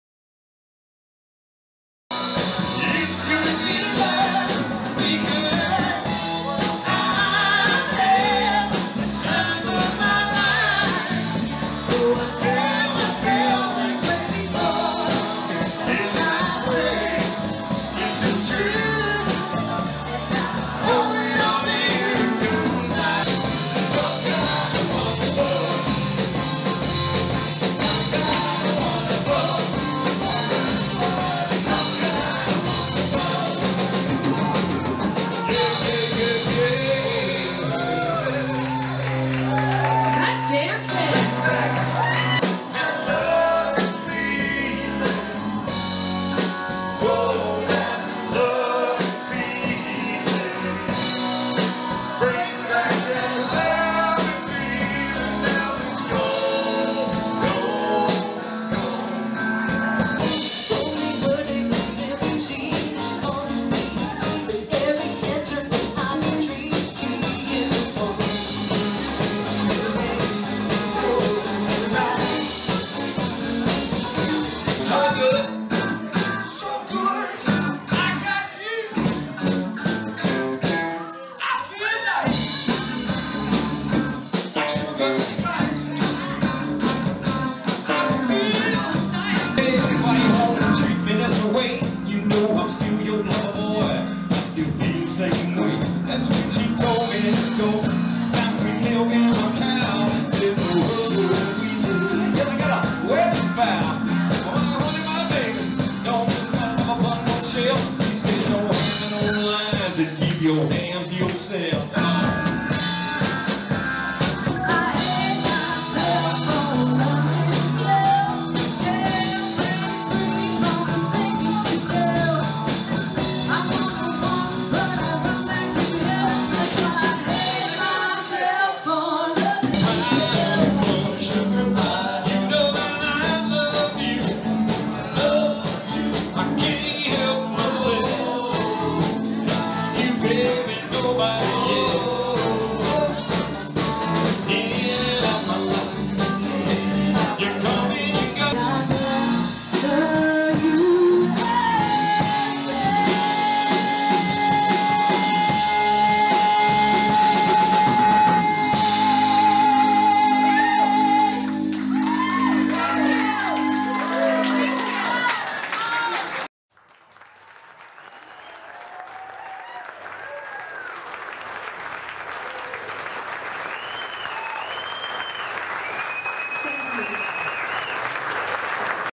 Reunion Virginia Band Video Sampler
Reunion Virginia is a dance band from the state of Virginia.  We play hundreds of songs...of all variety.  Perfect for Parties, Dances, Night Clubs, Festivals.